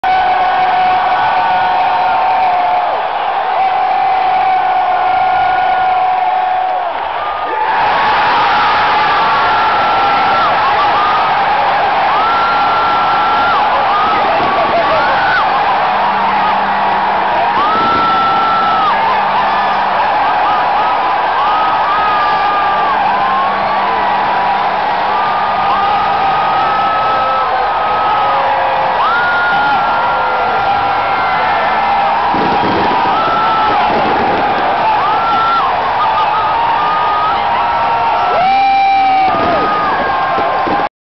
2008 World Series Final Out
Phils closer Brad Lidge strikes out pinch hitter Eric Hinske to win the World Series for the Phillies